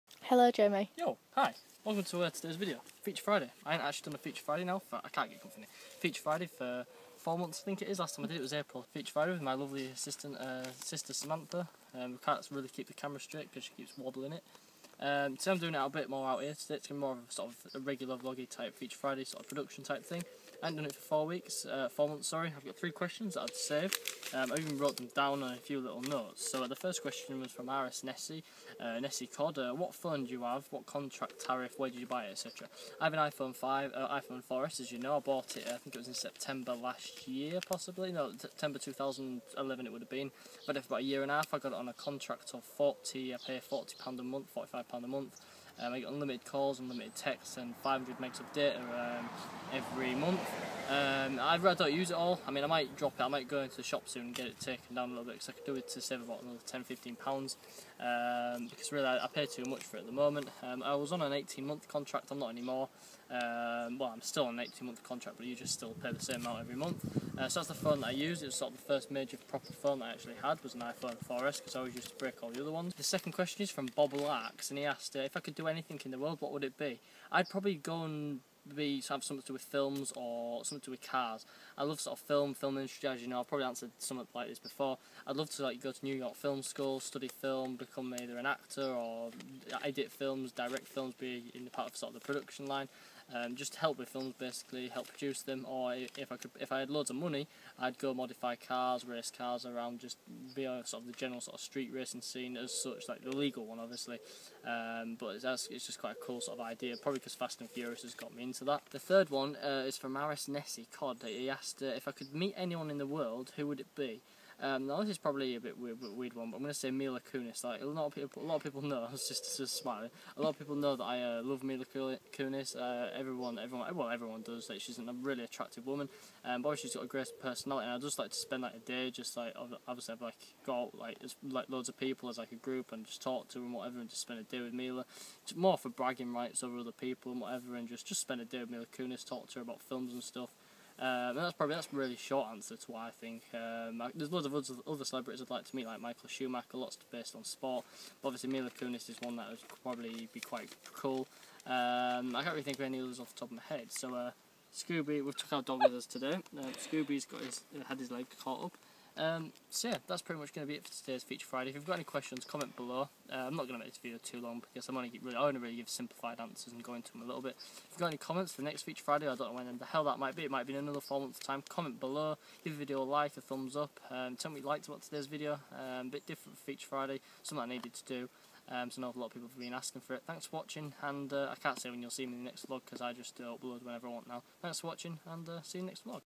Feature Friday #16 | Return After 4 Months | Mila Kunis | Phones | ItsJamieIRL | Daily Vlog #377
After a few people asking where is Feature Friday, here is the sixteenth episode, this time in a different location and three different questions recorded by my sister. If you enjoyed this please leave a question for next time.